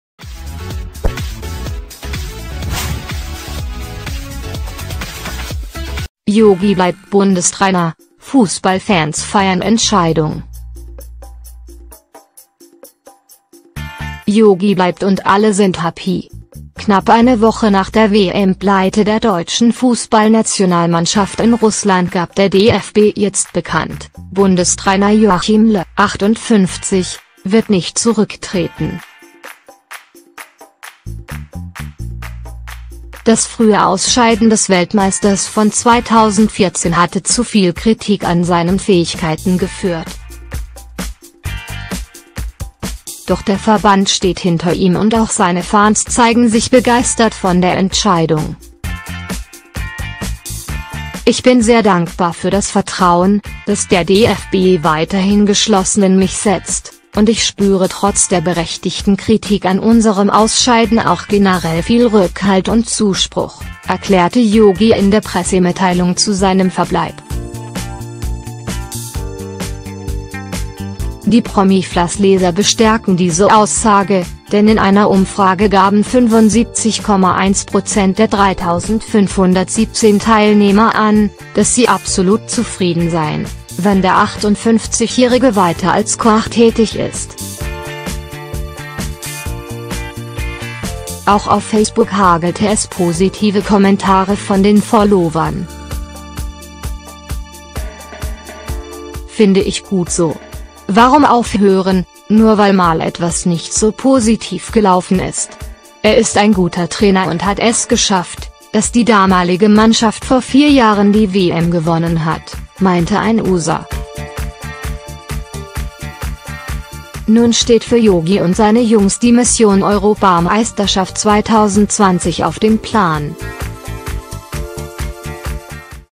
Jogi bleibt Bundestrainer: Fußball-Fans feiern Entscheidung!
Jogi bleibt und alle sind happy! Knapp eine Woche nach der WM-Pleite der Deutschen Fußballnationalmannschaft in Russland gab der DFB jetzt bekannt: Bundestrainer Joachim Löw (58) wird nicht zurücktreten. Das frühe Ausscheiden des Weltmeisters von 2014 hatte zu viel Kritik an seinen Fähigkeiten geführt. Doch der Verband steht hinter ihm und auch seine Fans zeigen sich begeistert von der Entscheidung.